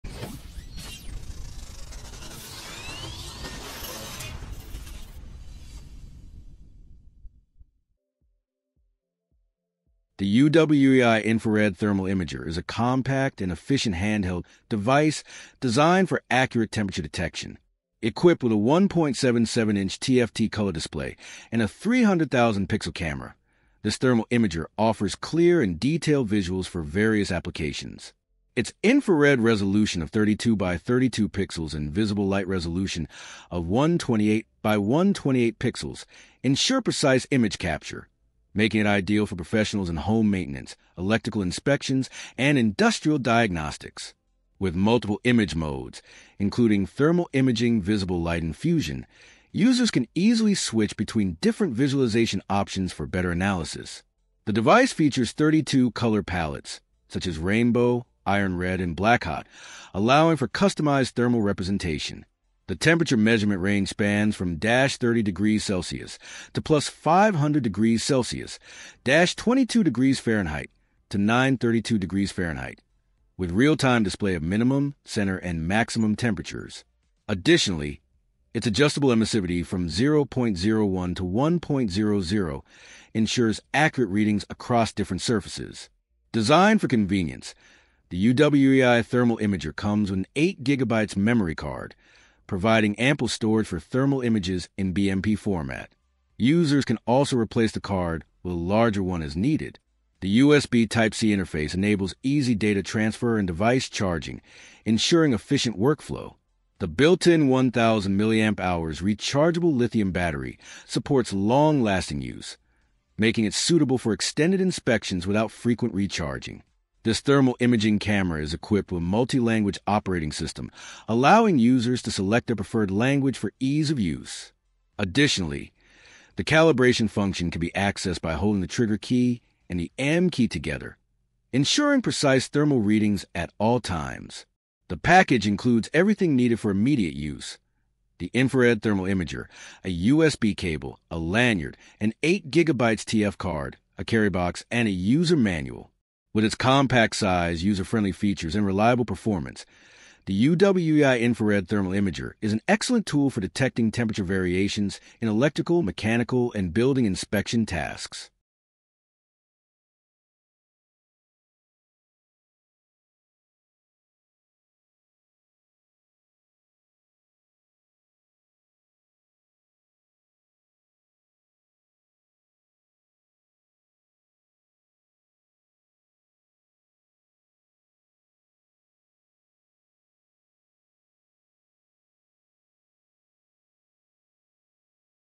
UWEI Infrared Thermal Handheld Imaging Camera Review Aliexpress
Specs UWEI Infrared Thermal Imager Handheld Thermal Imaging Camera USB Rechargeable 8GB Card Color Display 0.01-1 Emissivity Carry Box Review Aliexpress, The UWEI Infrared Thermal Imager is a compact and efficient tool for detecting temperature variations. Featuring a 1.77-inch TFT color display and a 300,000-pixel camera, it offers clear visuals with a 32 × 32 infrared resolution. Its multiple image modes, including thermal imaging, visible light, and fusion, allow for versatile use in various inspections.
With a temperature range of -30℃ to +500℃ (-22℉ to 932℉) and adjustable emissivity, it ensures accurate readings. The device includes an 8 GB memory card for easy data storage and supports USB Type-C charging for convenience. A built-in 1000mAh rechargeable battery allows extended operation.
Designed for professional and home use, the UWEI thermal imager is perfect for electrical diagnostics, mechanical maintenance, and building inspections. It comes with essential accessories, making it a practical and reliable choice for temperature analysis.